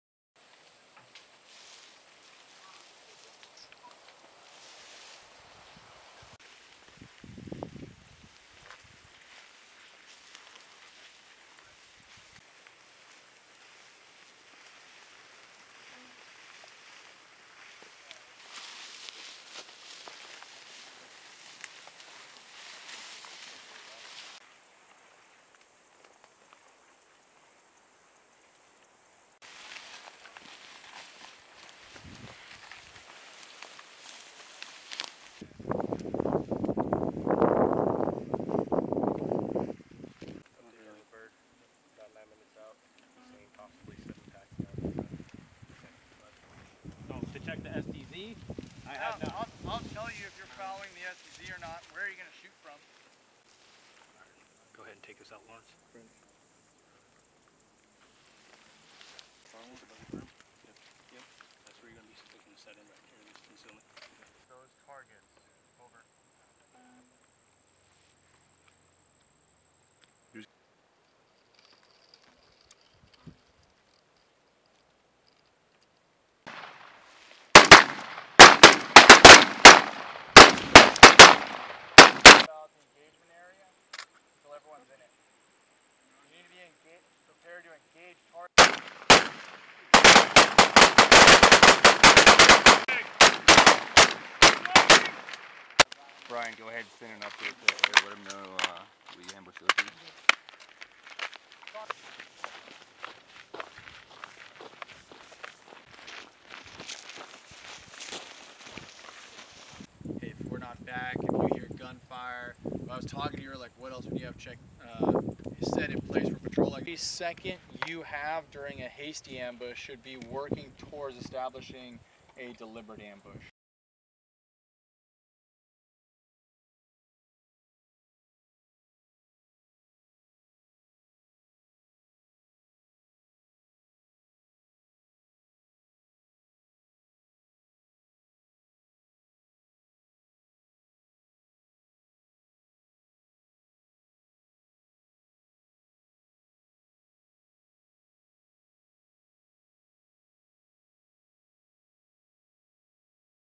B-Roll: 2d Recon Hasty Ambush Drills
CAMP LEJEUNE, NC, UNITED STATES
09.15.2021

U.S. Marines with Force Reconnaissance Company, 2d Reconnaissance Battalion, 2d Marine Division, conduct hasty ambush drills near Camp Lejeune, N.C., Sept. 14, 2021. Force Recon Co. conducted the drills to enhance their effectiveness in small unit warfighting techniques. (U.S. Marine Corps video by Sgt. Dylan Chagnon)

Film Credits: Video by Sgt. Dylan Chagnon
2nd Marine Division